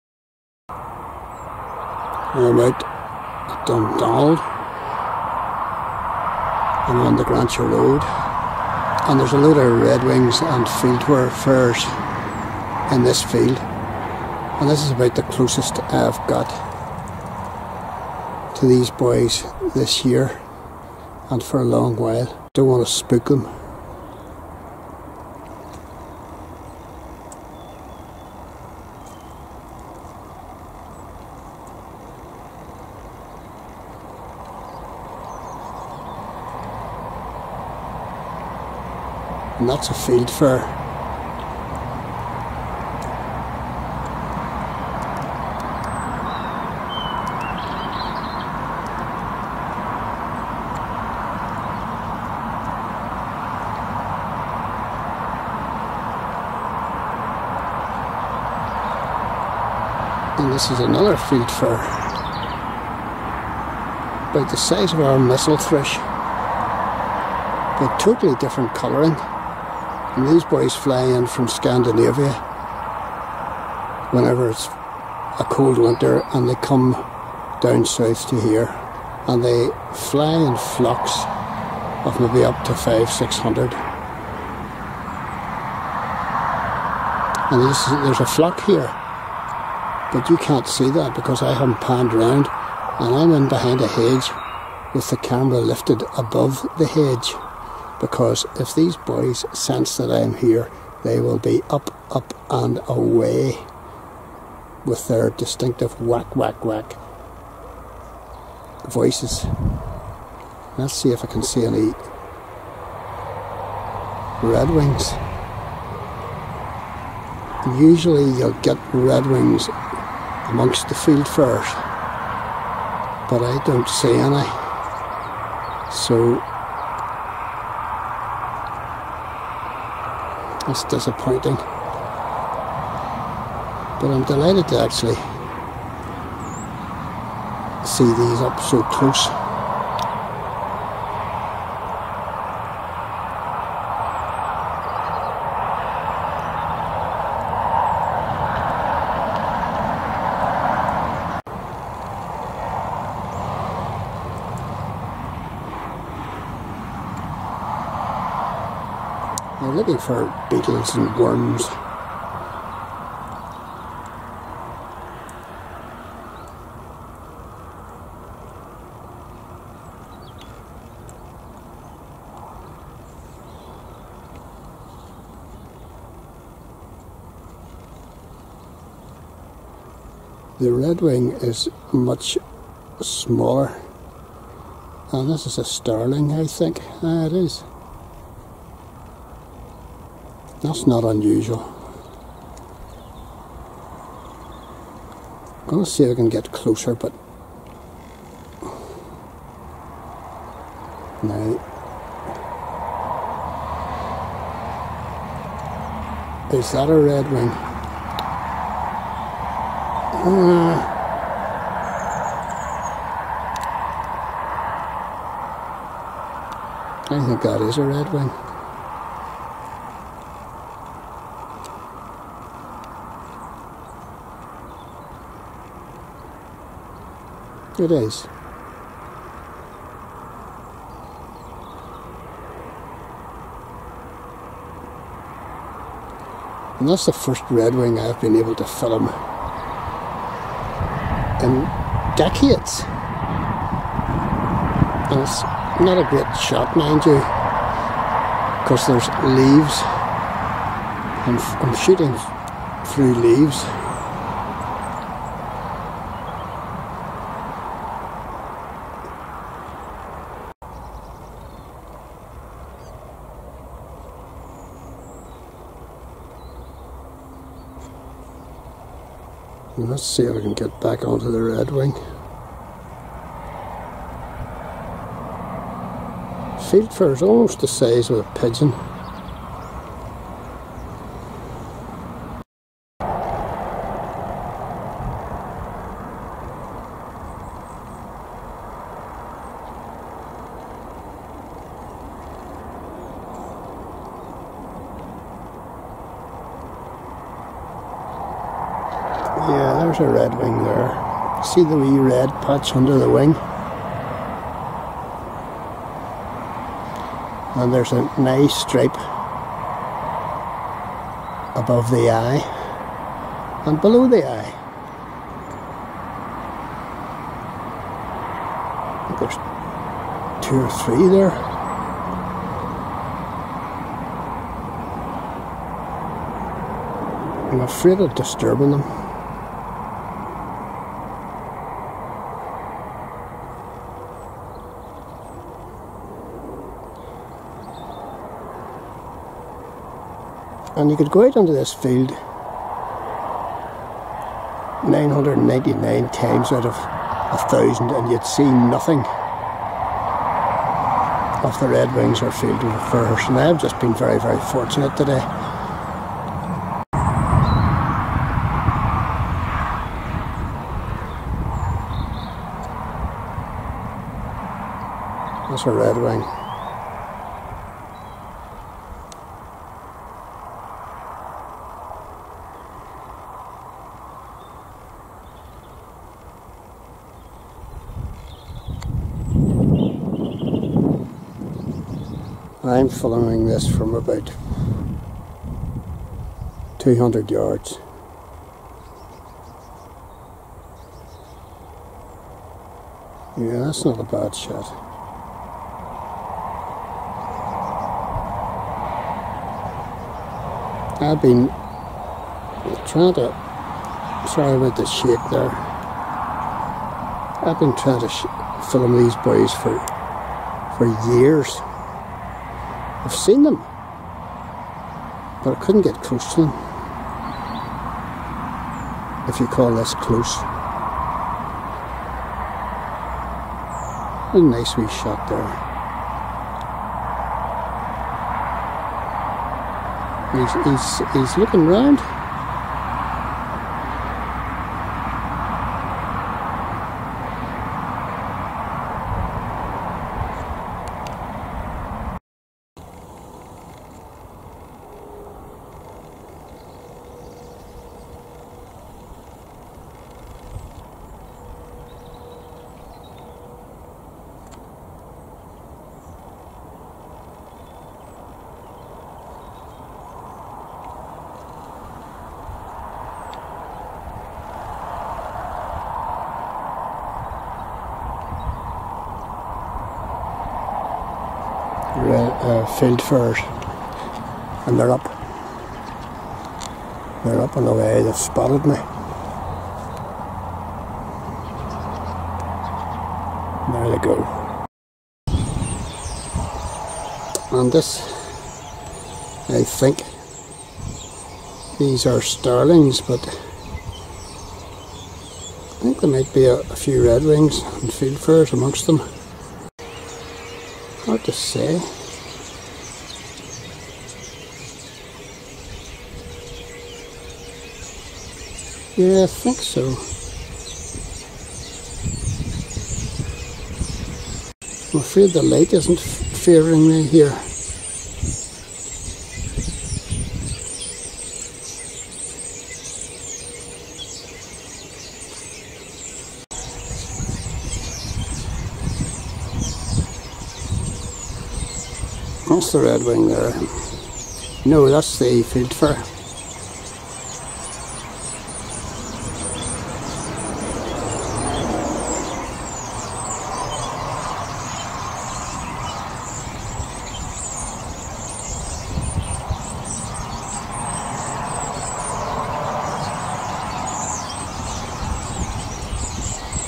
Trying to Film Fieldfares and Redwings near Belfast
I'm just off the Gransha road outside Dundonald Belfast.

I've been trying for years to film fieldfares and redwings from 'close range'. Yesterday I was delihted to stumble across a field full of these Scandinavian thrushes that arrive here for our milder winters.

I know that this is a rather poor film record. Fieldfares and redwings are extrememly difficult to film because they are so easily spooked. They willtake to the wing if you come anywhere near them. This film is the best I have managed to do so far. So I am well pleased.